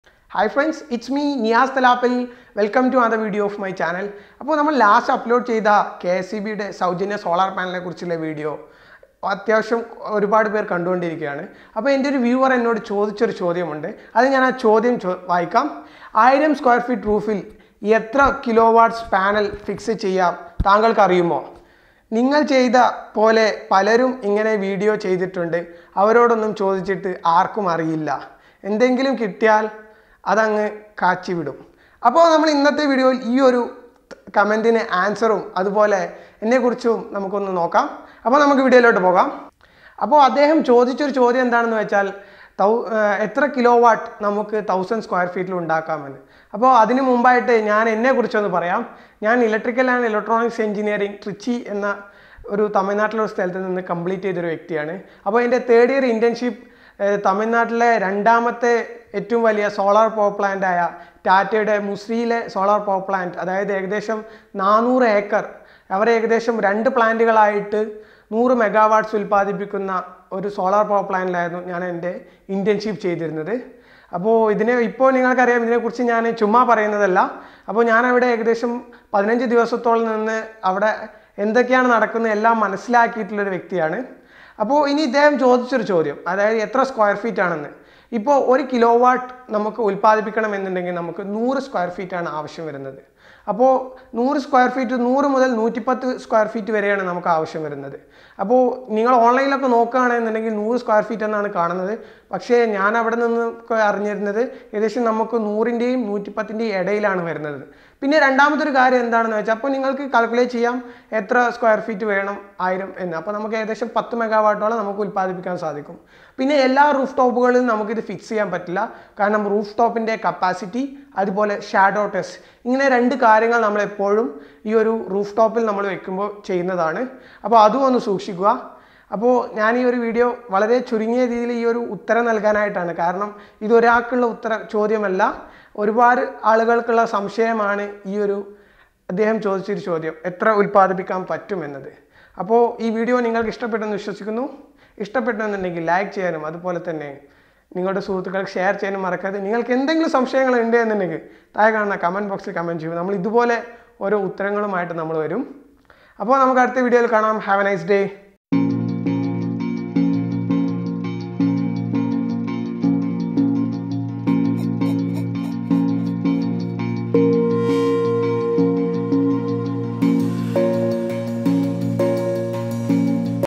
What is the cost of a rooftop solar plant in 1000 sq.ft, and how much power does it produce?
What is the cost of a rooftop solar plant in 1000 sq.ft, and how much power does it produce in Malayalam

Solar Power plant is one best Green Energy is available. This video explains solar power in Malayalam.

1000 sq ft area shall be enough for 12–13 kW system

However, the area should be shade-free... Also, note that panels will be sloping towards the south (for the northern hemisphere)

1kw system requires roughly 75–80 sq ft shade-free area... So you can calculate accordingly.

10 kW system should cost around 5 lac INR after govt subsidy

You get on average 4 units generated per kW (this is yearly average... You will get more in summer and less during cloudy days)

So 10 kw.. 4 units per day... So average you get 40 units per day ..multiply by 365 and you can get at least 14600 units in a year.

NOTE: All content used is copyright to Niyas Thalappil,